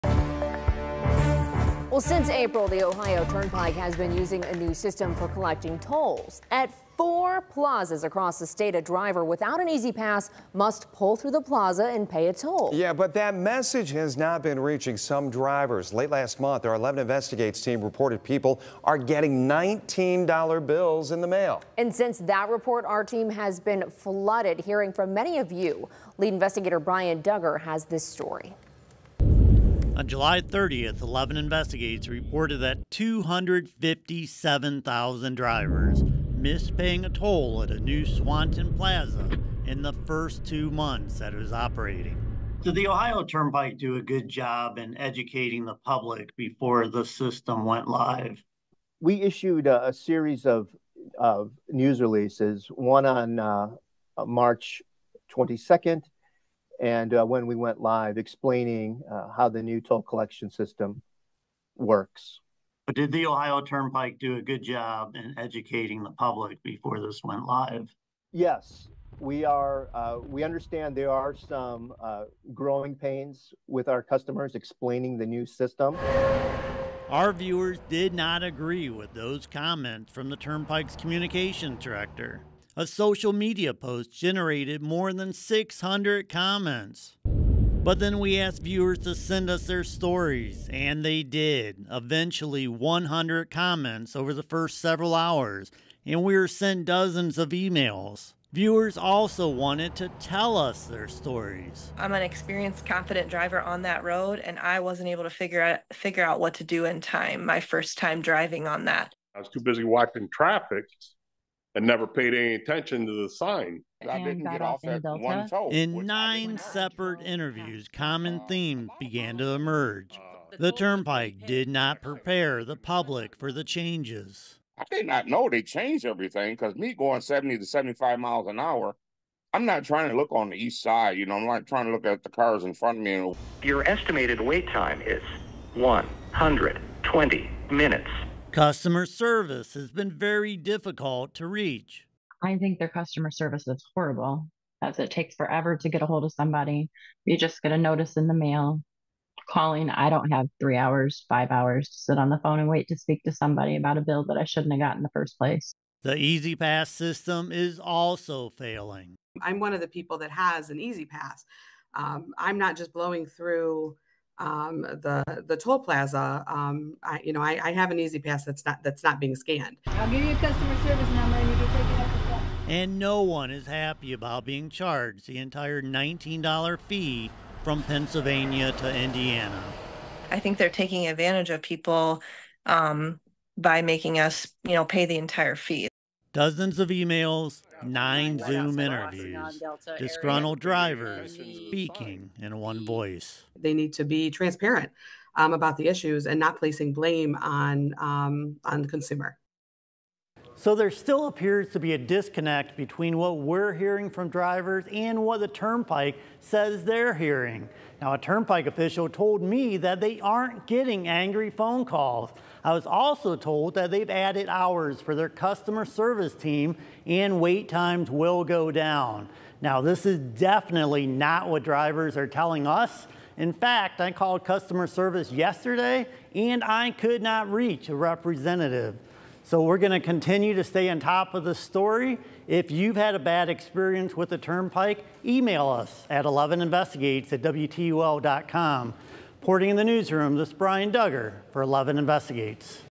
'I’ll probably never see my $20 again' | Drivers vent over bumpy Ohio Turnpike toll rollout
WTOL 11's posts received more than 800 comments, while dozens emailed and called 11 Investigates to complain about bills and an inability to reach customer service.